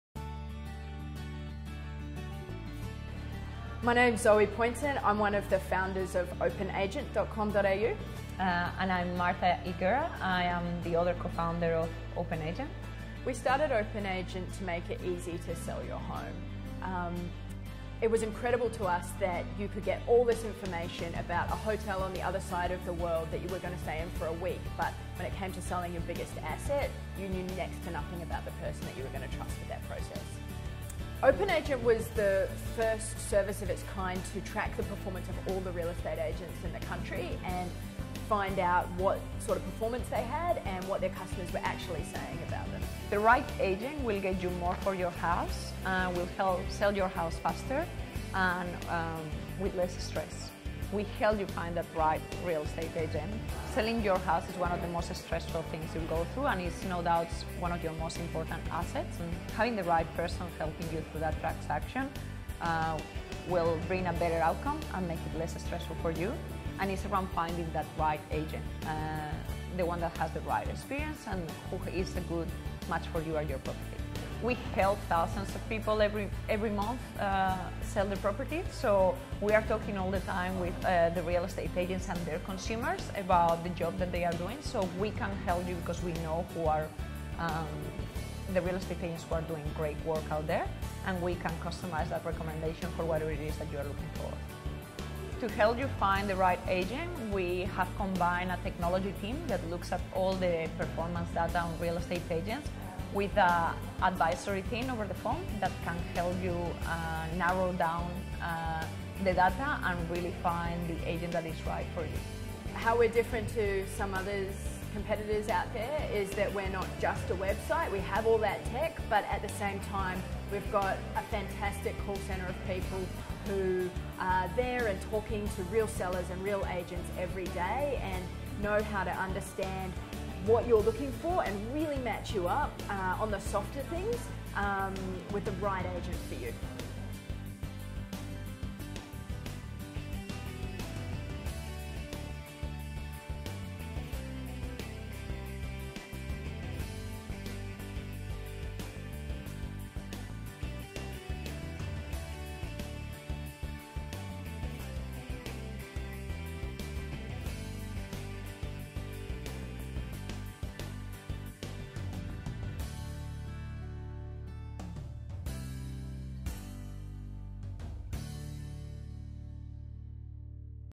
Do you know anyone who is selling their house soon? OpenAgent can help!
Do you know anyone looking to sell their house soon? Share this post with them to help them find out how OpenAgent can help sell their house faster, smarter and for more money.

OpenAgent compare thousands of real estate agents throughout Australia to find an agent that suits your needs. OpenAgent is a 100% free, no obligation service.